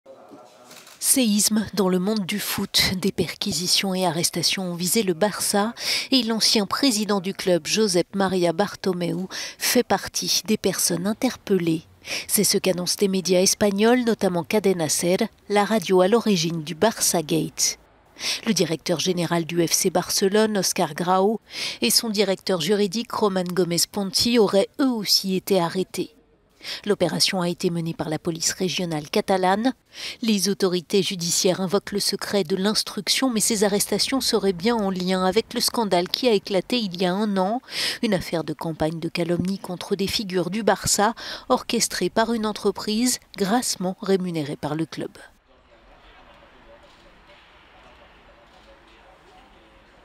Perquisitions et arrestations au Barça
L'ancien président du FC Barcelone, Josep Maria Bartomeu, fait partie des personnes interpellées, selon les médias espagnols.